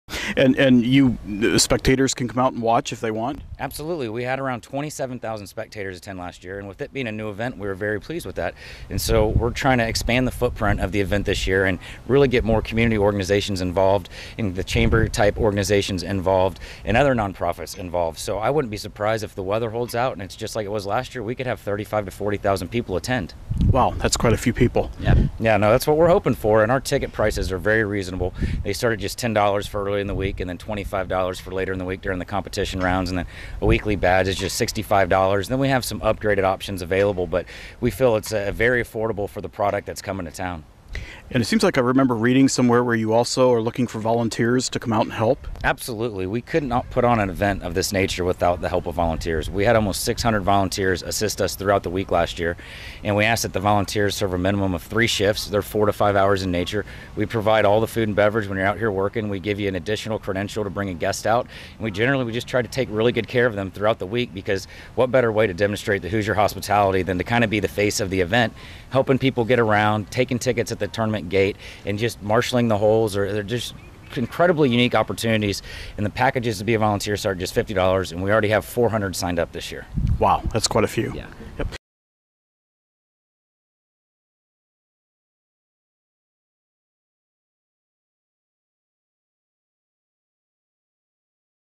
2014/05/31: Duke Butler IV on the Hotel Fitness Championship, segment 3
On May 31, 2014, we talked with Duke Butler IV, tournament director, about the Hotel Fitness Championship.

In this segment, we talk about spectators and volunteer opportunities at the tournament.